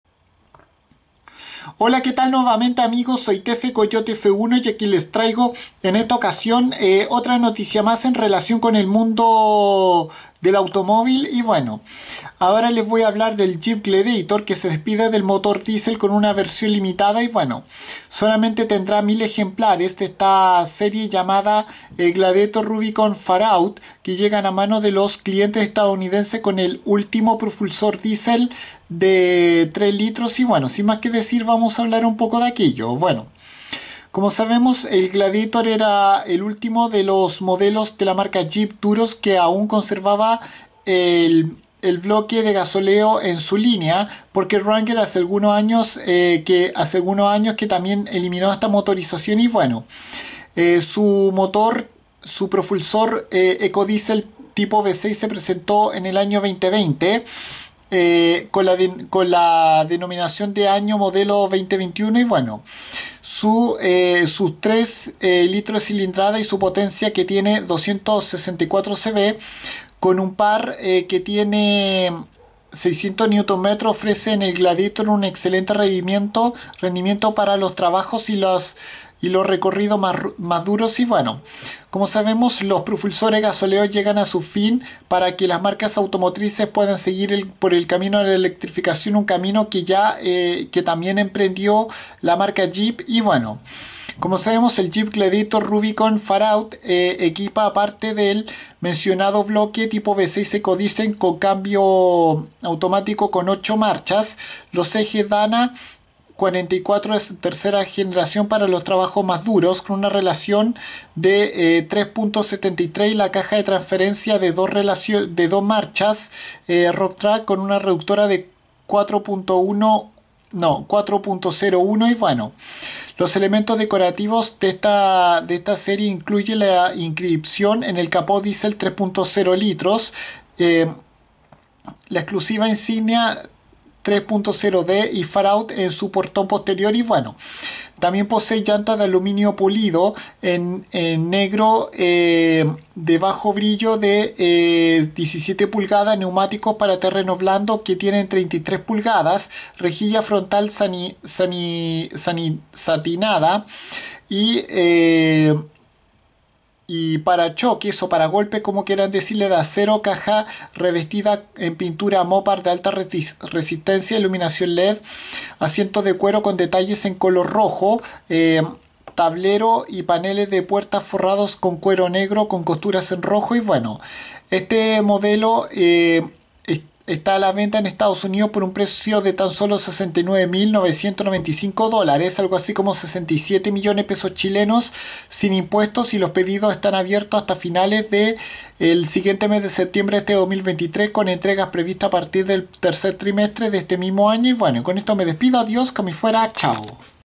Jeep Gladiator Rubicon FarOut información
Esta es una versión especial limitada que solo tendrá mil ejemplares y que despide a la variante de motor diésel